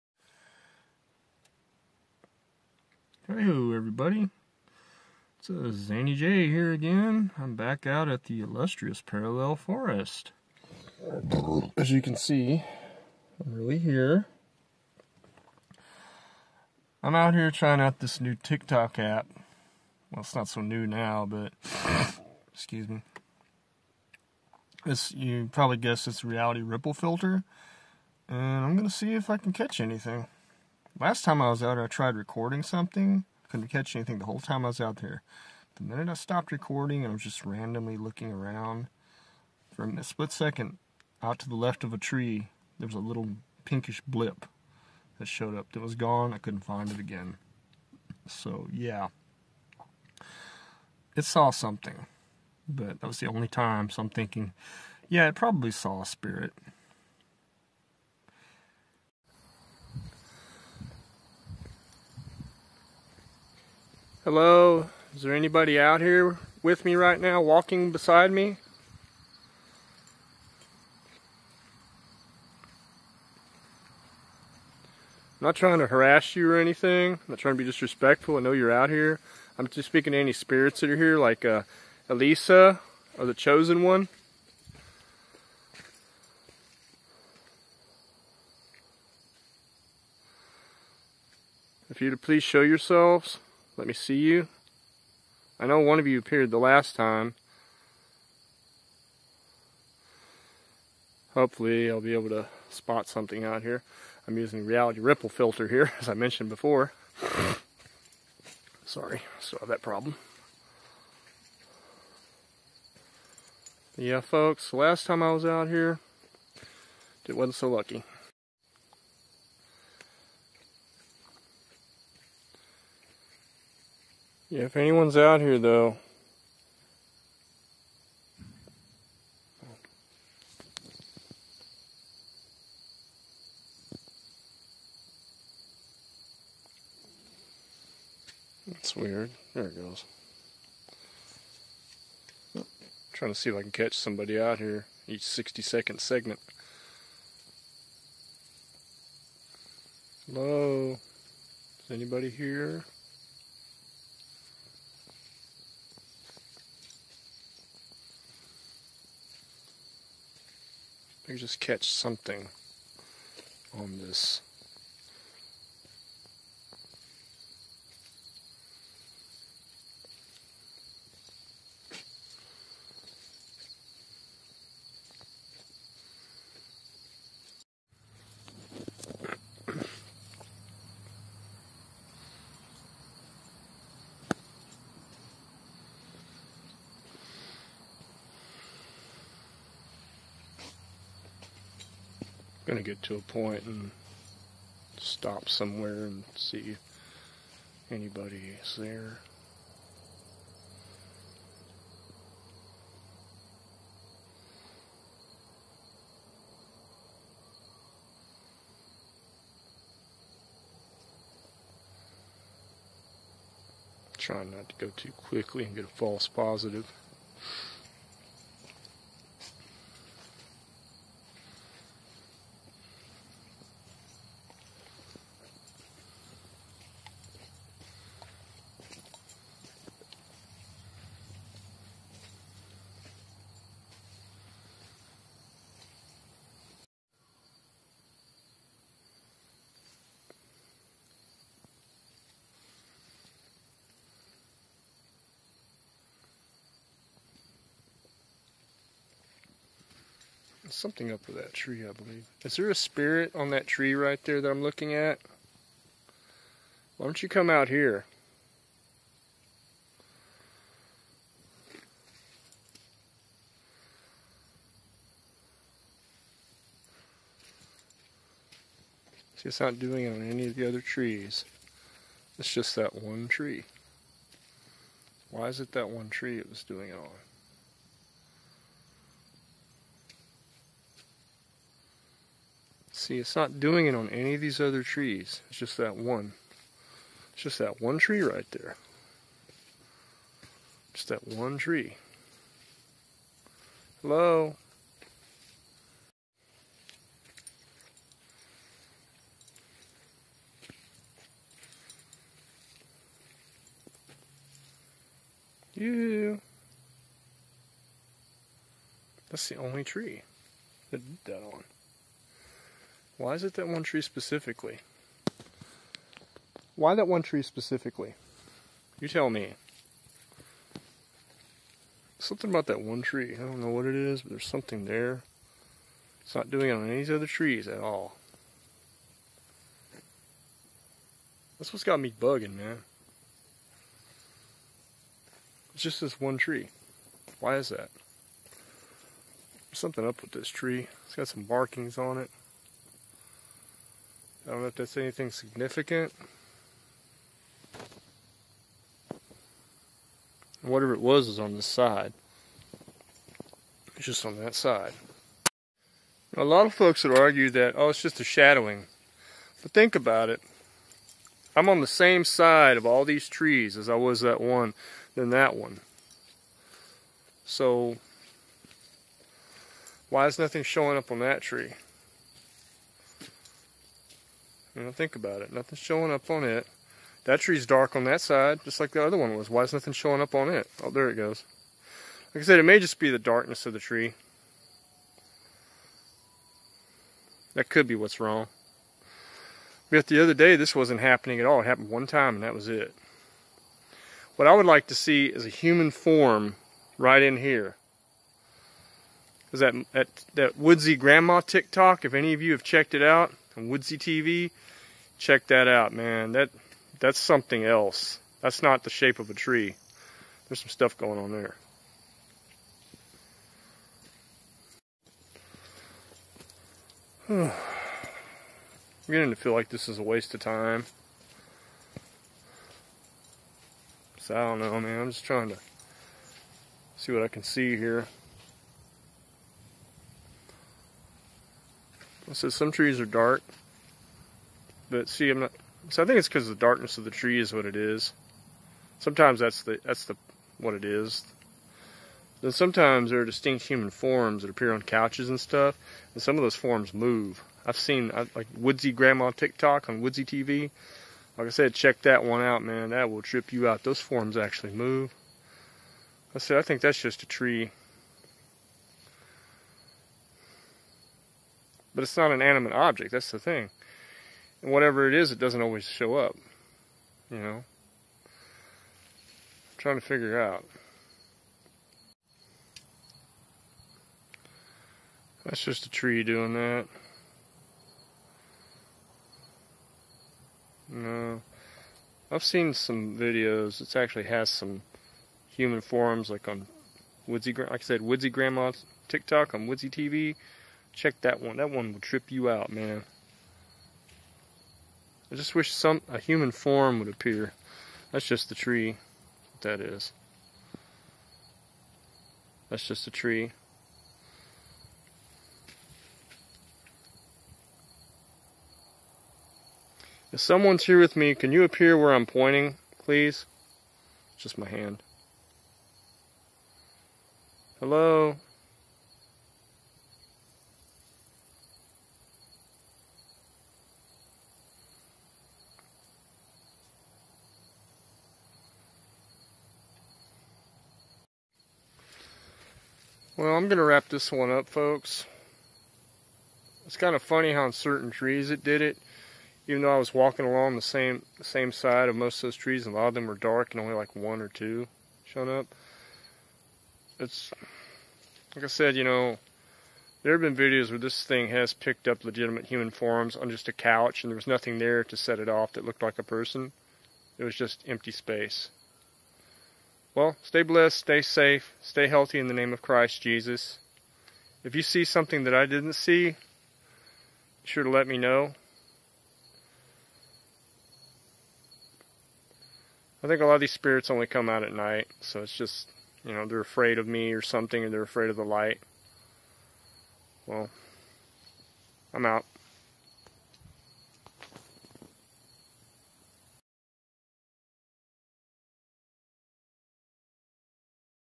TikTok Parallel Forest Visit
Trying out the Reality Ripple Effect Filter in the Parallel Forest...